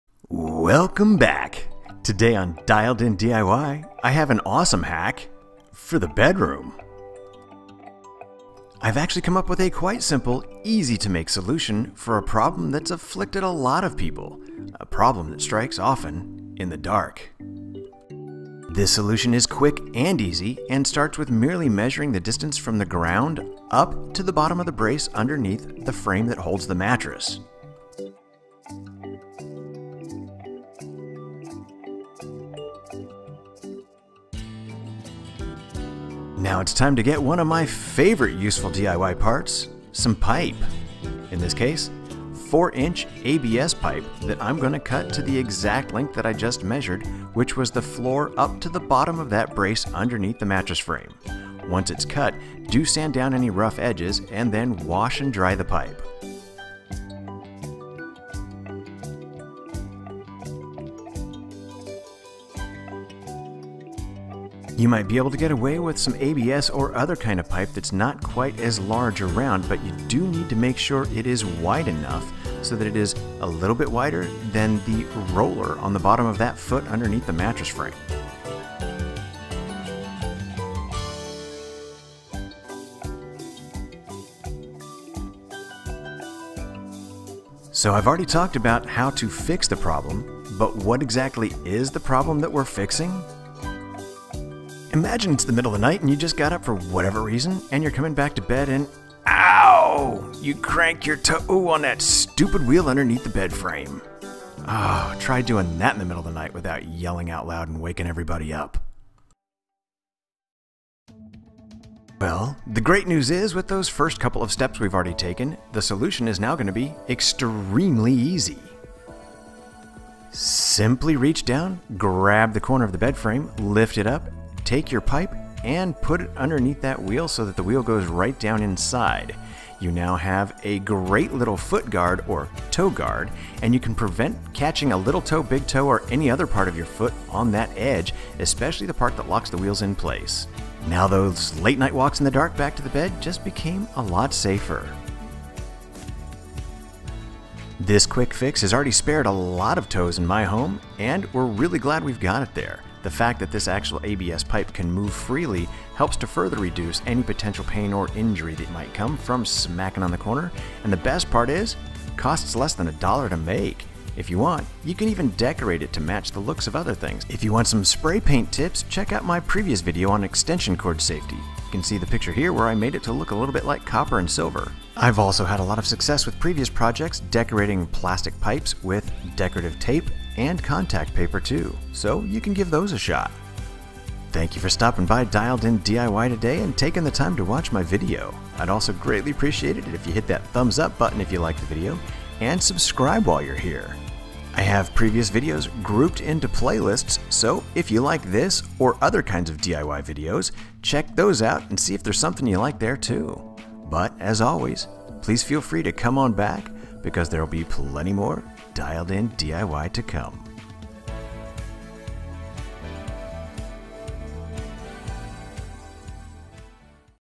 Hack Your Bed - Easy & Awesome Toe Saver
This really cool bedroom hack is easy to do and definitely reduces painful experiences in the bedroom.  In just about 10 minutes and roughly $1 you can do what we have done to get rid of some shockingly painful bedroom incidents!  Hack, safety tip, DIY solution, practical mod; call it what you want but IT WORKS.

In this video I used 4" ABS pipe that you can find in just about any hardware store in lengths as short as 18" and up to 8 - 10'.  I get the longer pieces because the cost per foot is much less.

I also mentioned decorating tips (spray painting plastics) from this previous video - "Power Cord Safety - DIY Security for Extension Cords"